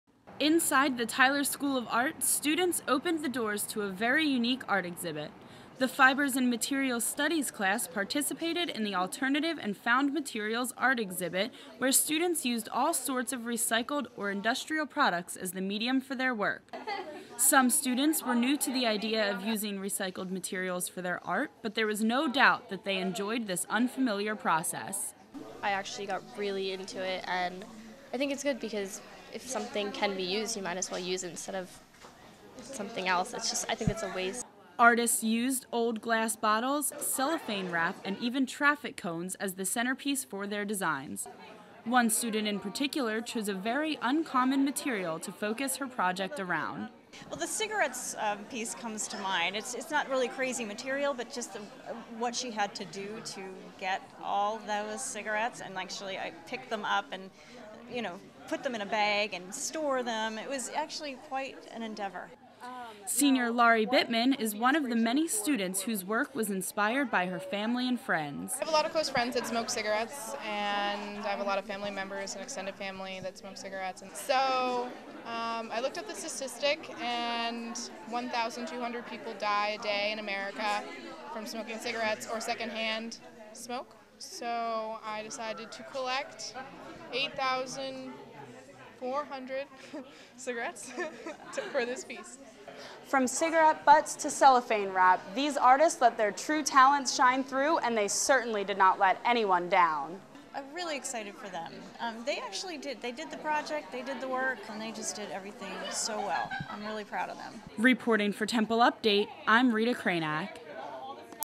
Tyler School of Art Alternative Materials Exhibit
Temple University's Sustainability Week came and went, but these Tyler School of Art students used the week as an inspiration for new art.

Reporter: Rita Kraynak
Photographer: Rita Kraynak